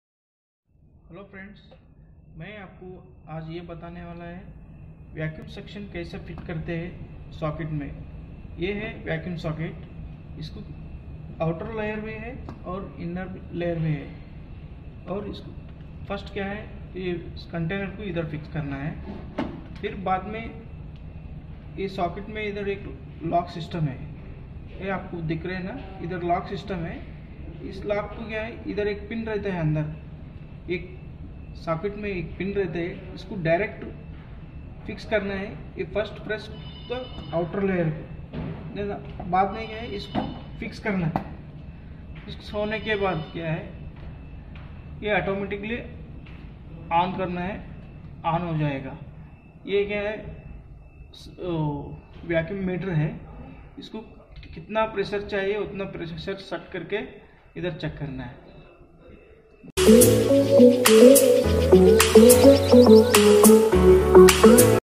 Fix and check vaccum unit.
How to fix vaccum unit in the vaccum socket easily.